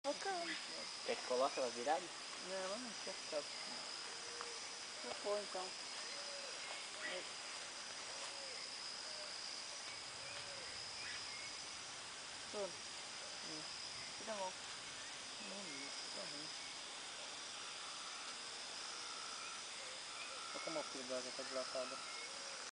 Perereca macaco (Phyllomedusa tetraploidea) no Parque Estadual Mata dos Godoy - LONDRINA
Monkey frog (Phyllomedusa tetraploidea) in Godoy State Park - LONDRINA

A Perereca macaco (Phyllomedusa tetraploidea, Pombal & Haddad, 1992) é uma das belas espécies de Anfíbios da família Hylidae encontrada em áreas alagadas no PEMG. Apresenta habito arbustivo e só é observada em ambientes florestais.